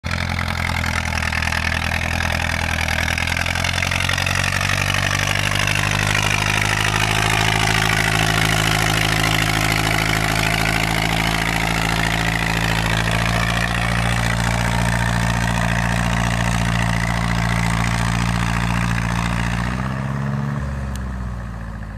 Fordson Super Major
Tilling with a Fordson Super Major.

Denmark